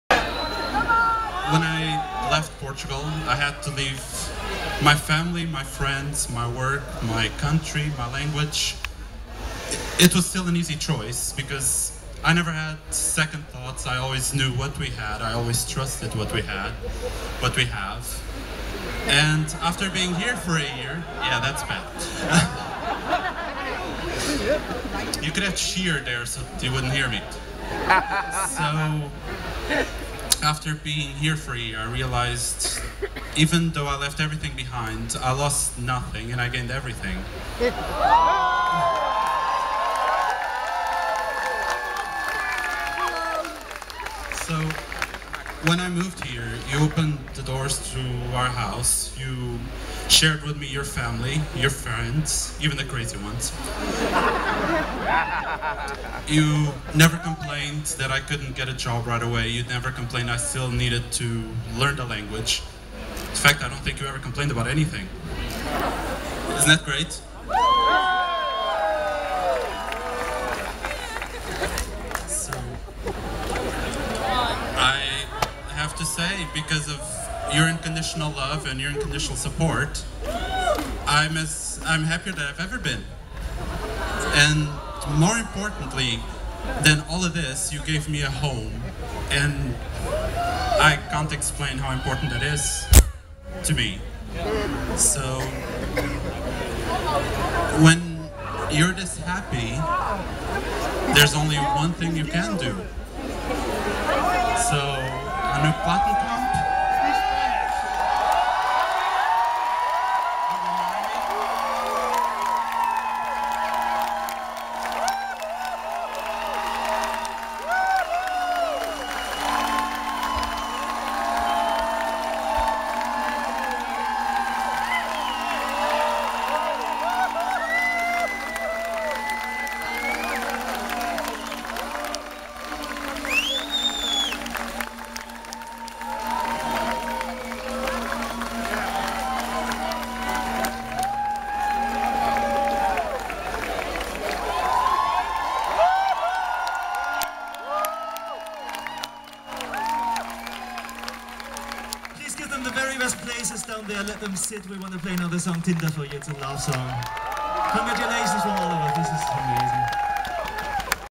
Castlefest 2013, festival of love. Huwelijksaanzoek bij het concert van Faun
Scherpstelling haperde zo nu en dan een beetje